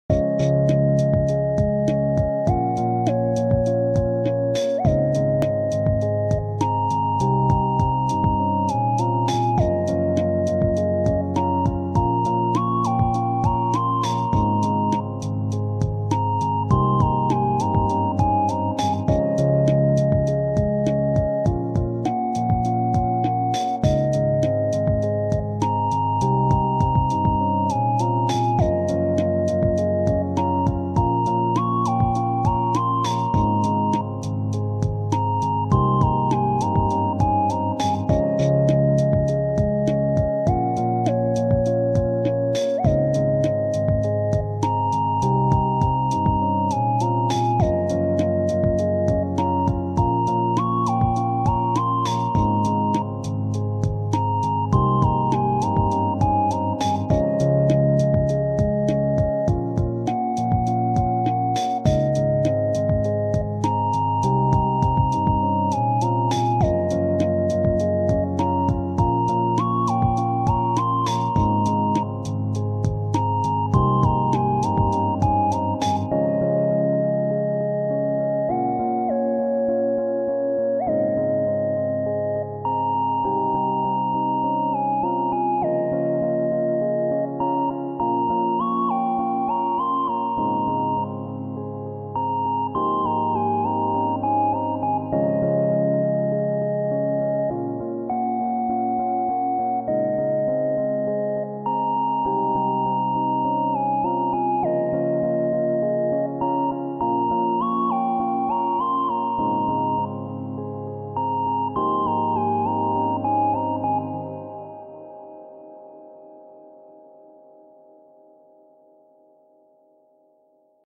Musique Libre De Droit Calme
Musique libre de droit calme pour vidéos YouTube et pour utilisation commerciale. J'ai fait cette musique sans droit d'auteur calme, douce et chill. Elle est gratuite sans copyright. Idéale comme musique de fond calme et posée.